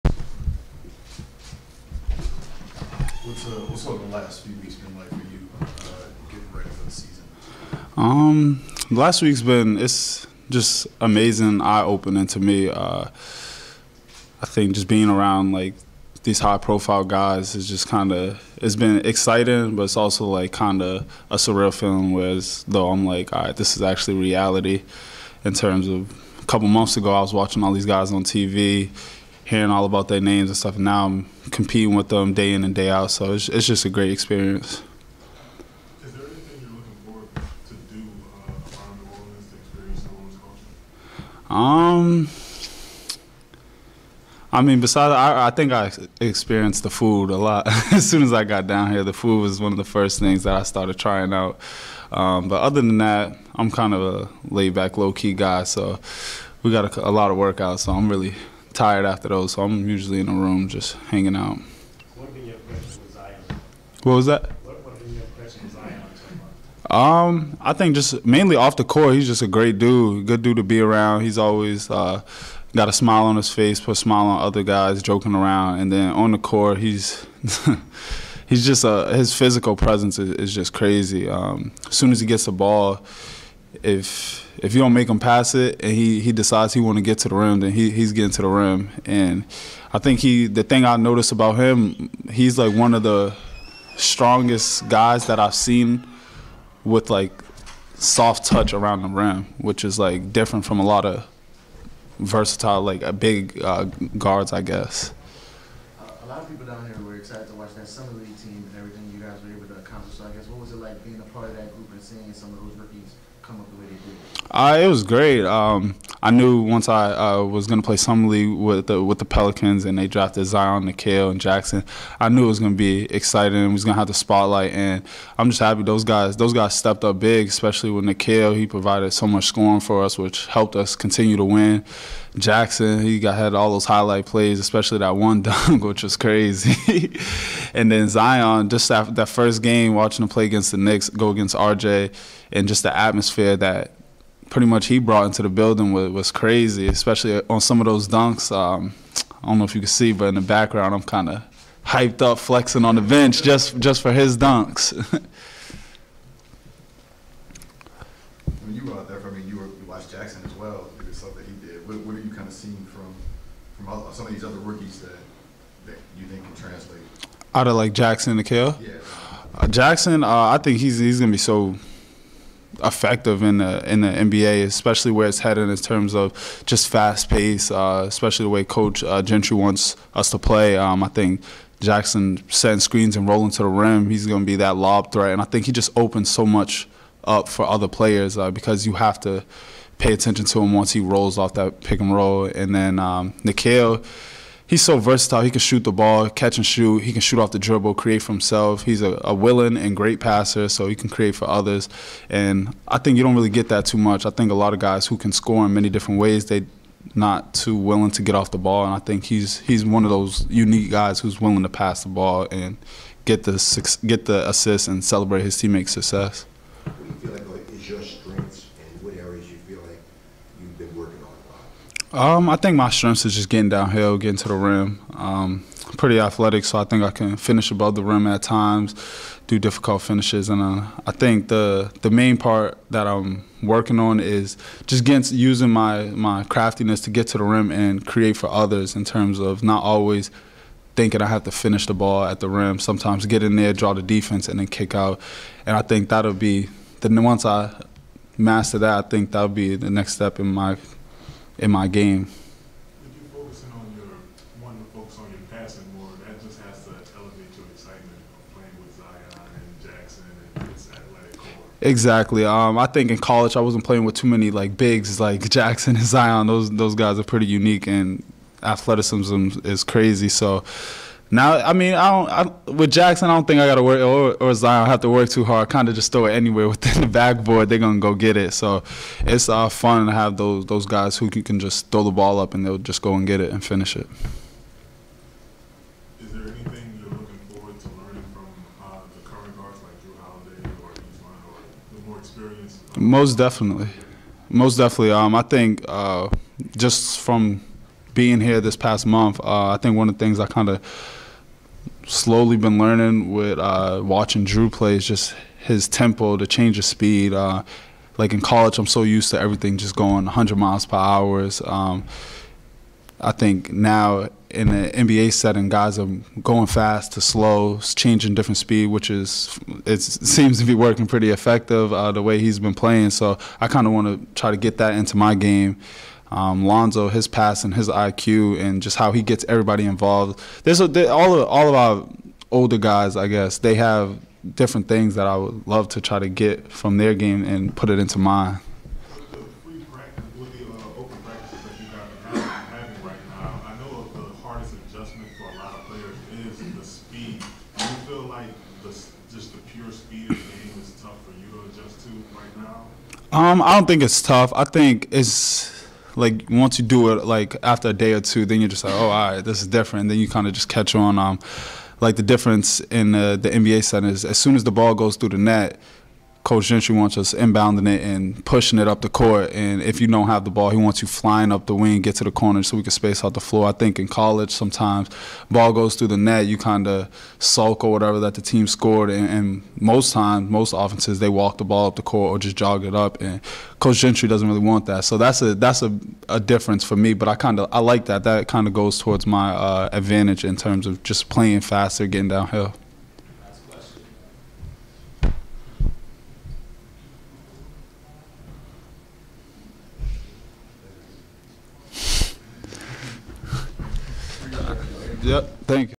Jalen Adams 2019 New Orleans Pelicans Media Day - Monday, Sep. 30, 2019
New Orleans Pelicans guard Jalen Adams addressed the media at 2019-20 Pelicans Media Day on Monday, September 30, 2019.